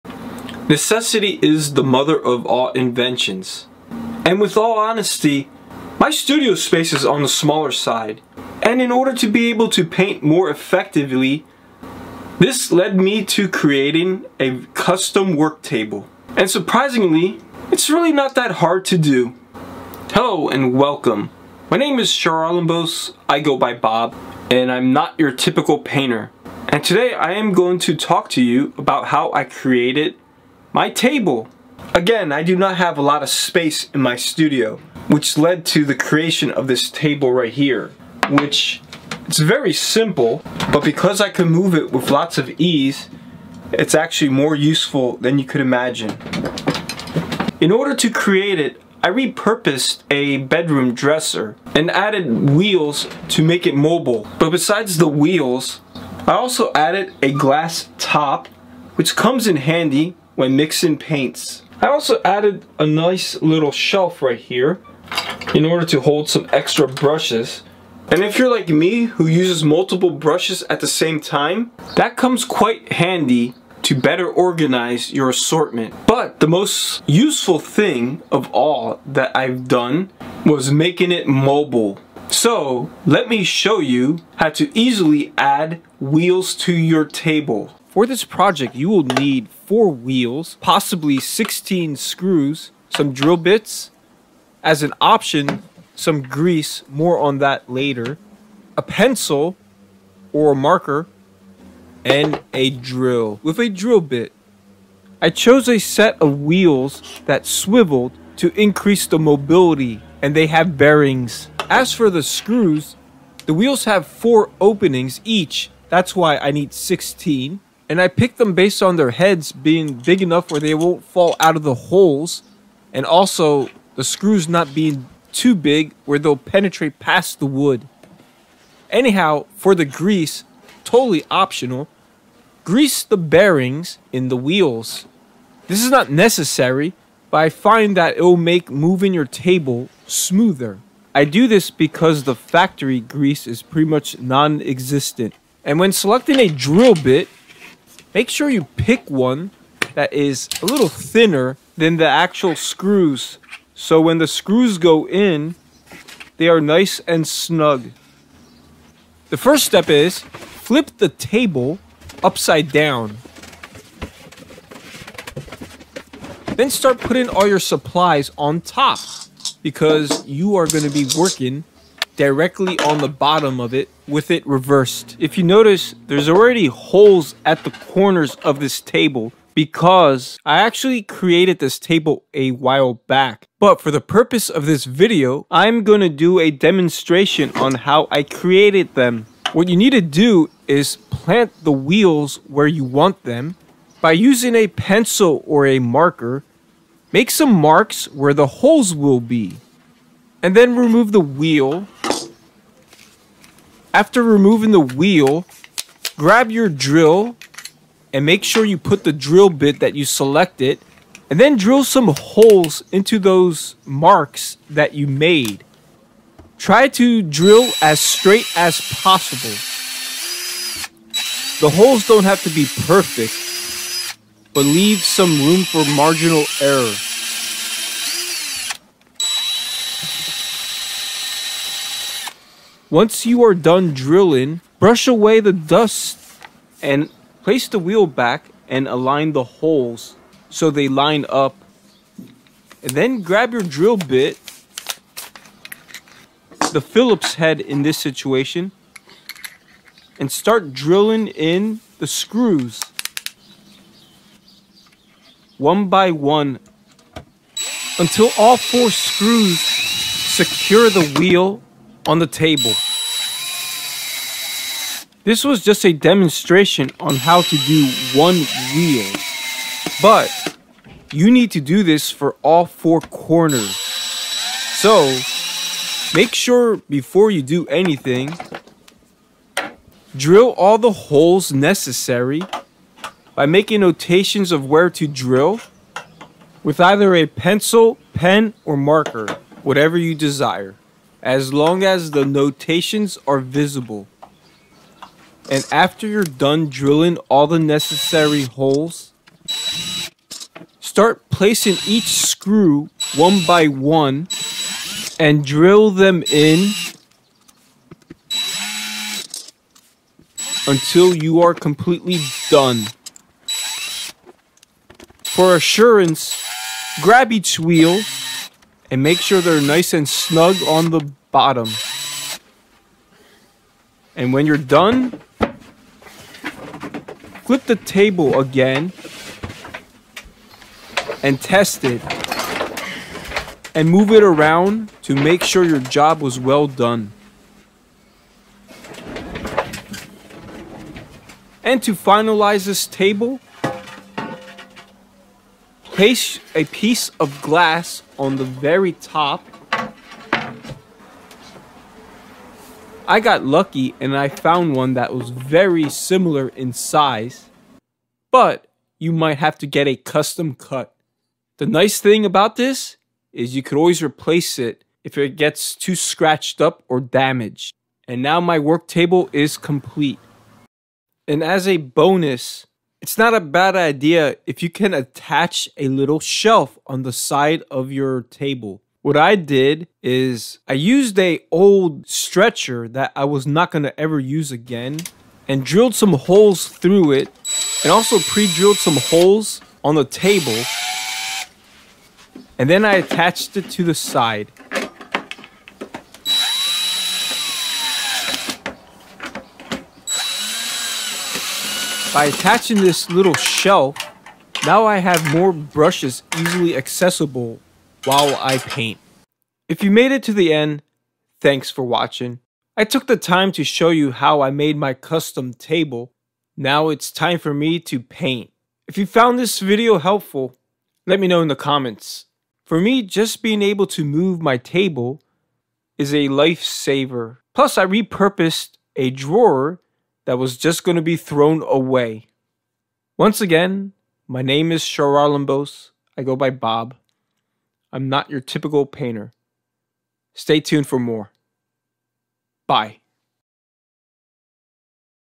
Simple Easy Small Spaces Studio Hack How To Make Your Own Custom Art Oil Painting Work Table Station
I thought it might be a good idea to share how I created my art table!

To make a long story short, I had the drawer with a piece of glass on top for a while! But one day while moving it around to adjust the area of the painting I was working on, I came up with a great idea! Why not add wheels to my table so it is more easy to move!

And this is very handy considering my studio is tiny!

Full disclosure, everything I used was from salvaged things so if you try to replicate this table it will be near impossible! The goal is to give you guys an idea of add wheels to a useful table so moving things becomes easier!

I bought the wheels and screws from Home Depot and already owned the drill and drill bits! As for the glass, I can see it being necessary to get a custom cut if your table unless you get lucky like me and find one that fits! The shelf on the side was a rack my fiancee did not need and was going to throw away!

Questions? Let me know int the comments!